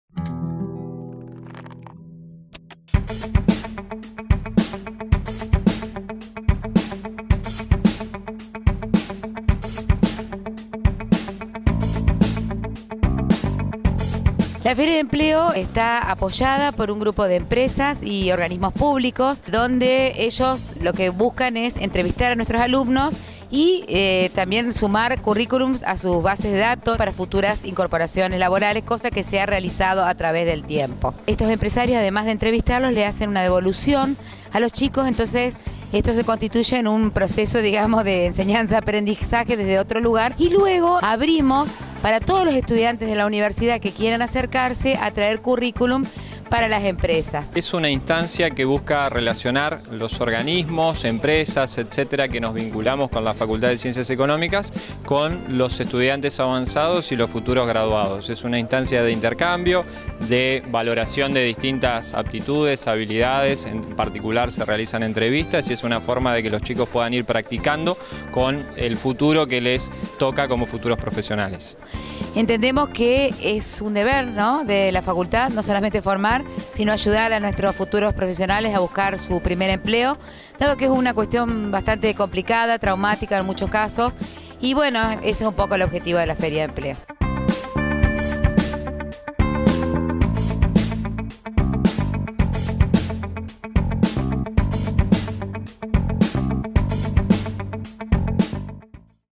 Feria de Empleo en Ciencias Económicas
El viernes 27 de octubre se realizó la séptima edición de la Feria de Empleo en la Facultad de Ciencias Económicas, en Paraná.

Los alumnos y graduados de todas las carreras y facultades de nuestra Universidad pudieron participar de charlas sobre estrategias, competencias y herramientas necesarias para la inserción y el desarrollo laboral.

A su vez, pudieron entregar sus CV a importantes empresas de la región.